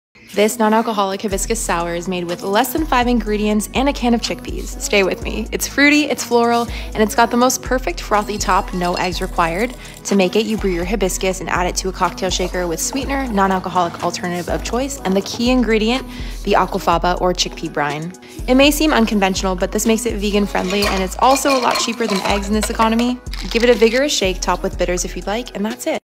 Hibiscus Sour Mocktail (no eggs!)
This frothy drink is floral, fruity and vegan friendly, made with aquafaba (chickpea brine) in place of eggs.

You need 5 simple ingredients to make it, plus a can of chickpeas.

YOU NEED:
⊹ dried hibiscus
⊹ non-alcoholic alternative (I used @newlondonlight)
⊹ simple syrup or liquid sweetener
⊹ fresh lime juice
⊹ aquafaba or chickpea brine
⊹ alcohol-free bitters (I used @allthebitter from @boisson.sips)

Have you ever tried an aquafaba based drink before?

Recipe →

⊹ main camera body
⊹ camera lens
⊹ vlog camera

discounts
⊹ Mate the Label → code VEGGIEKINS20